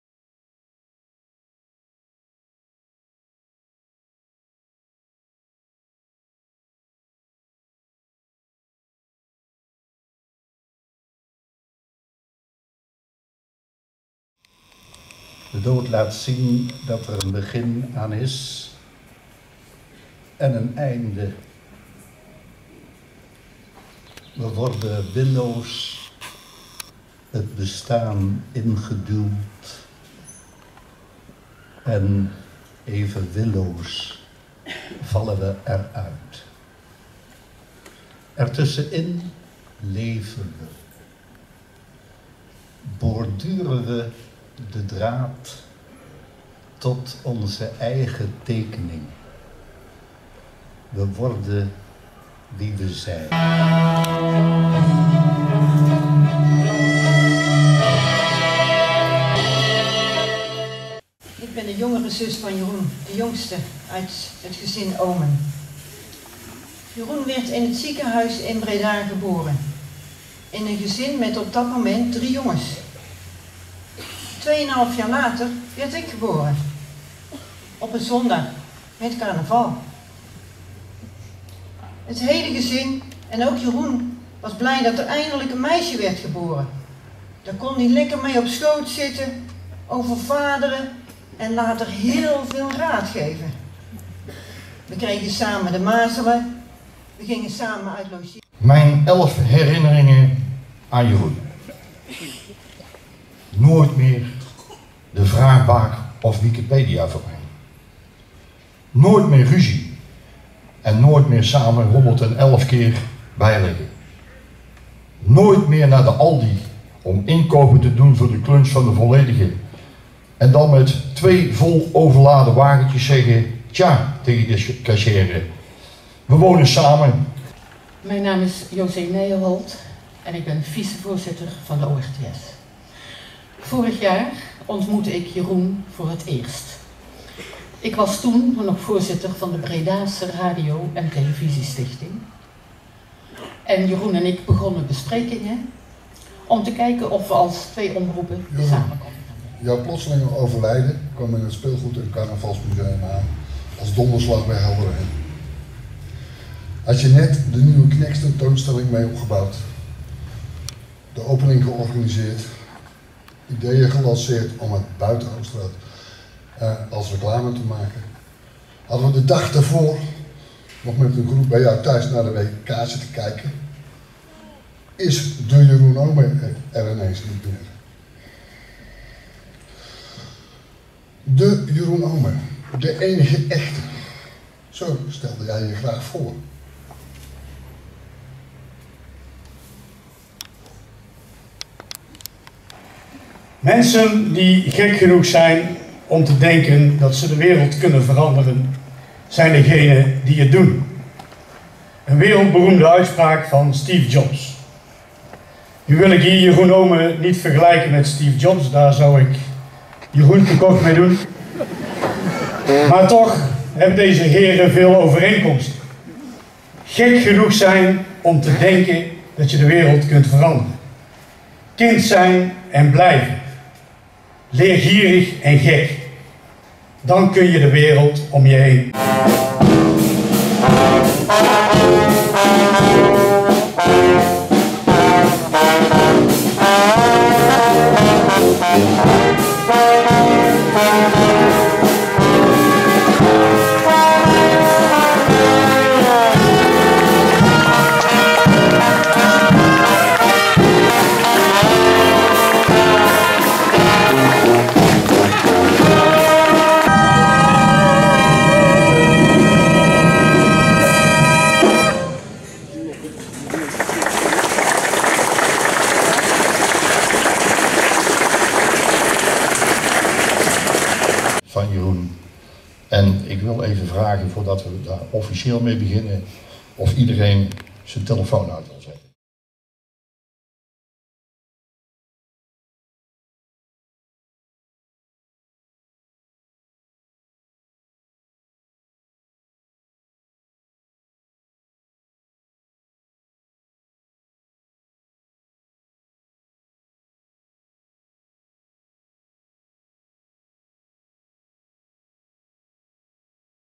Heemkundekring "De Heerlijkheid Oosterhout". "Afscheid van Jeroen Oomen"
Korte samenvatting van het afscheid van Jeroen Oomen in de "Schelp" Oosterhout , 17 juli 2014